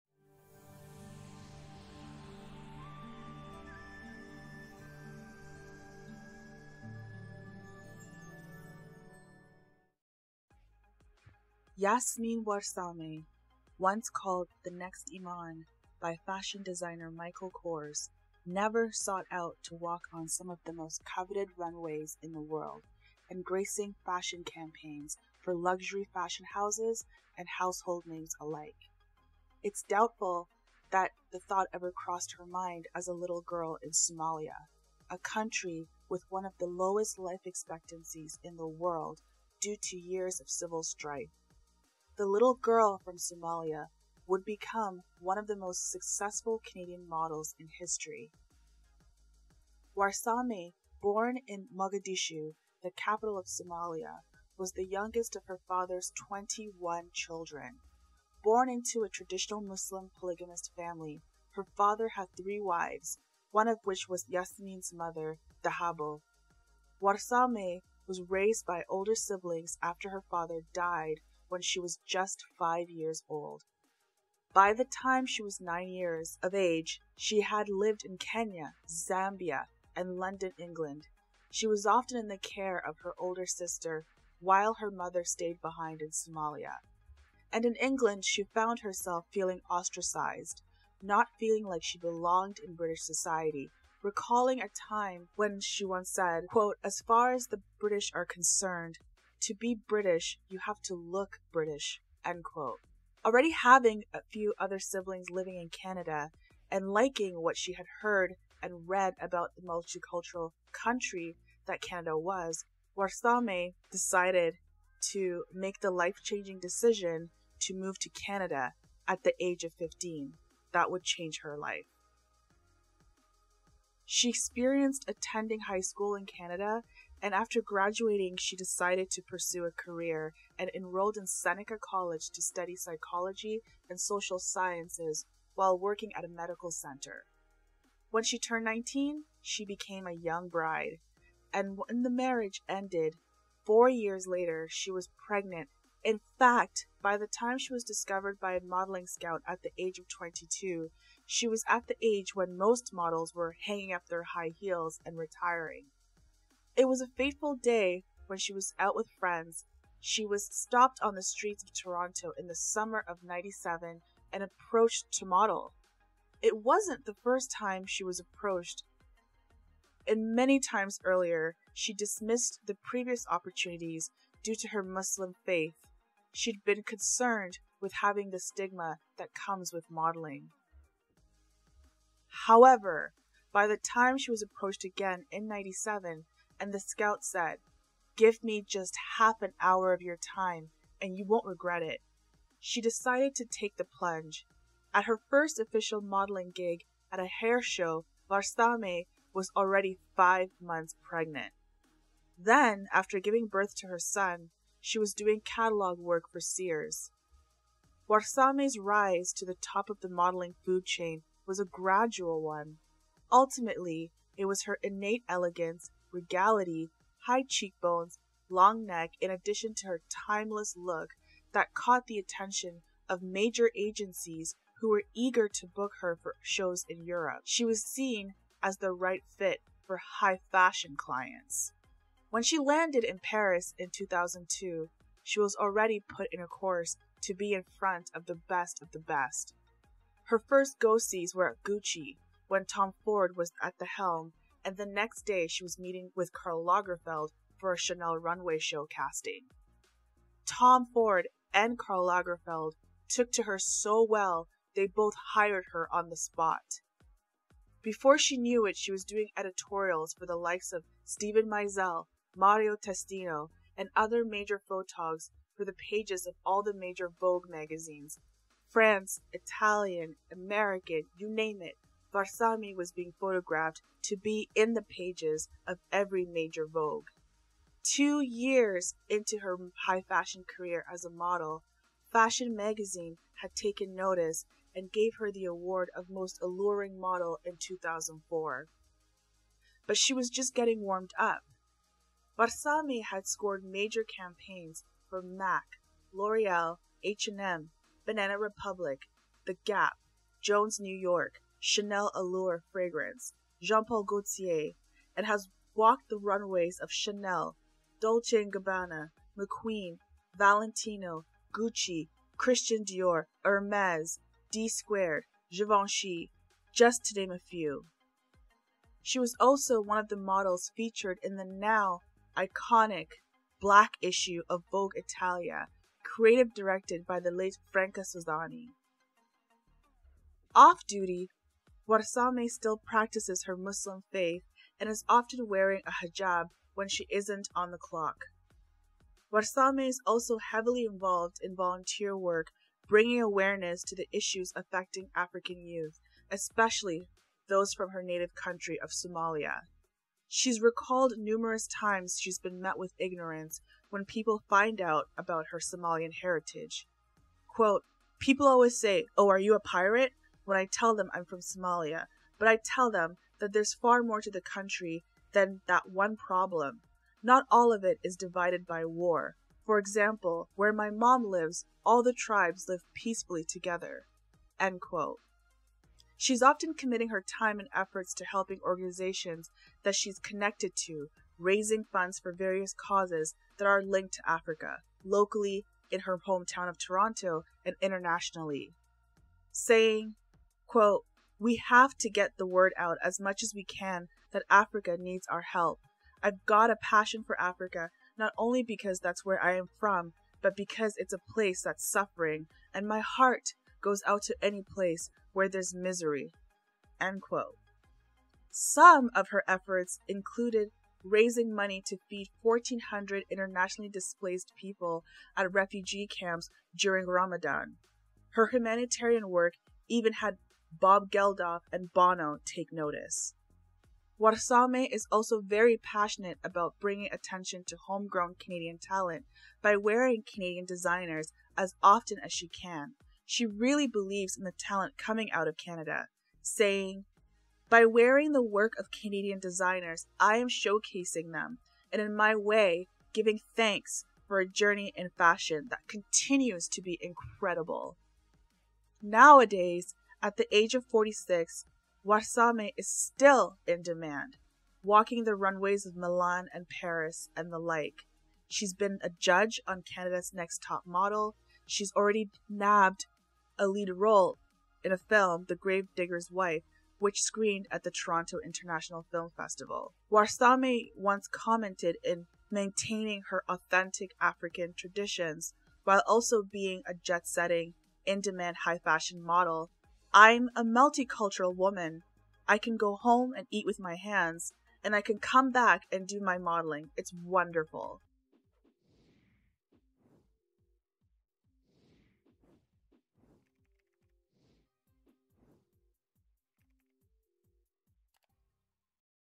Yasmin Warsame | The Timeless Beauty Called 'The Next Iman'
Yasmin Warsame, the stunning Canadian Supermodel who hails from the "Horn of Africa" and voted 'Most Alluring Model' by Fashion Magazine, frequently referred to as Iman's protégé, has been featured in coveted magazines, runways and global beauty campaigns ever since she left Toronto for Europe in the early 2000s.